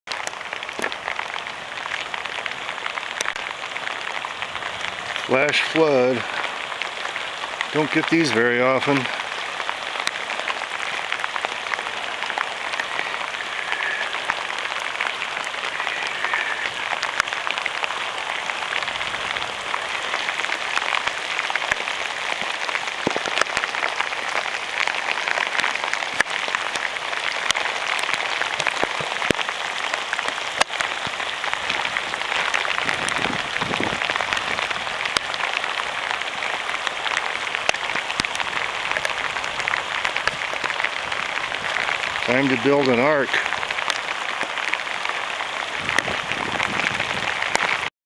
Flash Flood 7/20/21
We go months without rain and then get this!  Pretty rare to see rapids in our back yard.  All that greenery is just from the last few weeks.  Before the end of June it looked dry as the Moon out there.